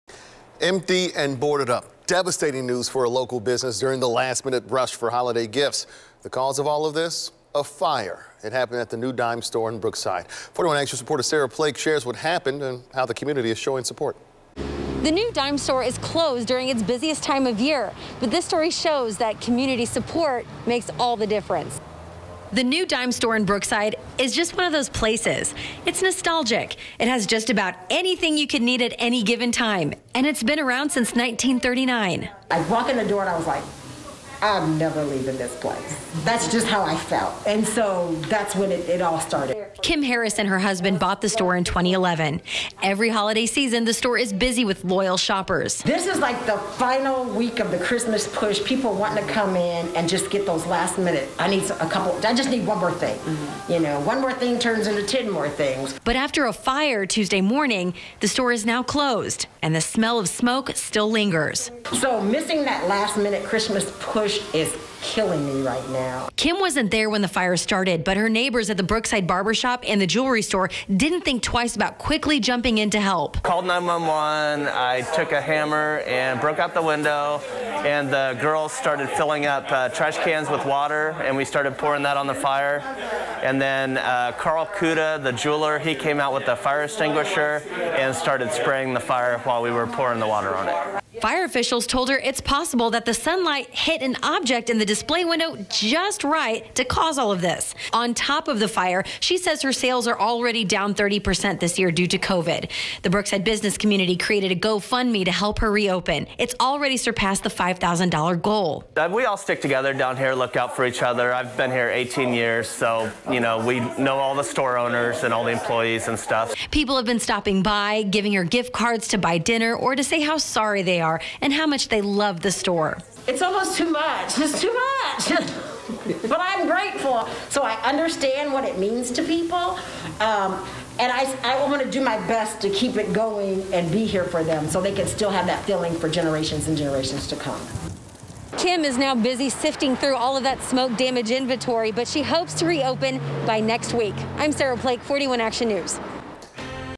Brookside community rallies after fire damages Dime Store
The community is supporting Kim Harris, the owner of The (New) Dime Store in Brookside, after a fire Tuesday morning.